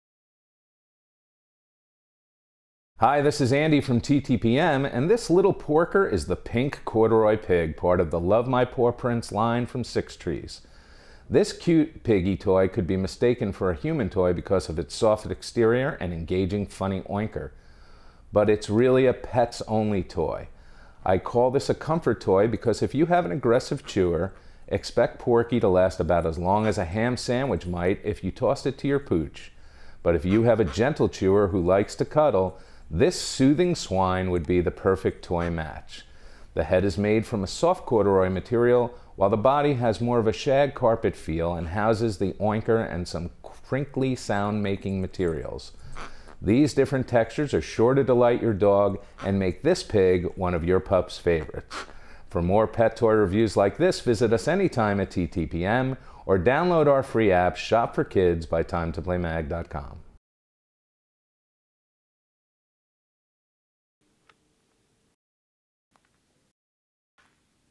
Pink Corduroy Pig from Sixtrees
The Pink Corduroy Pig has a soft corduroy head with a shag carpet body that houses an oinker and crinkle sound-making material.  For full review and shopping info